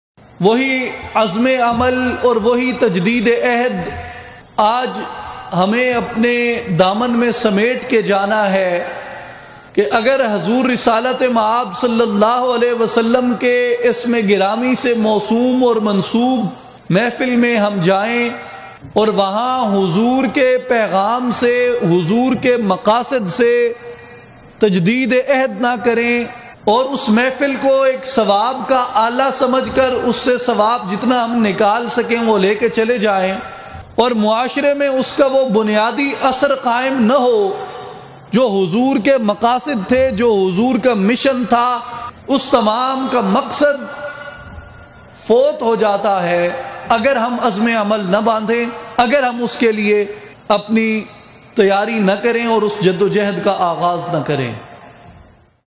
Purpose of congregation of Melad e Mustafa SAWW.
[ Explained By: His Excellency Sahibzada Sultan Ahmad Ali Sb ]
In annual Melad e Mustafa SAWW & Haq Bahoo Conference, organized by Islahee Jamaat & Aalmi Tanzeem ul Arifeen, at Sheikhupura on 4th February 2012.